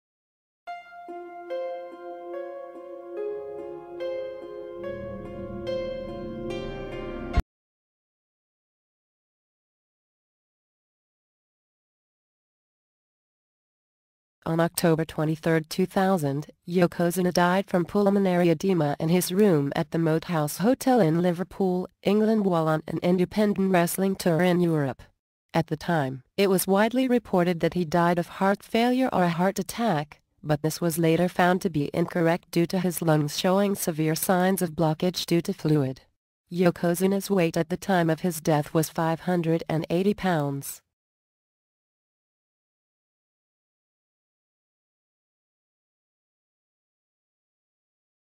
THE DEATH OF YOKOZUNA
This video details the death of wrestler Yokozuna!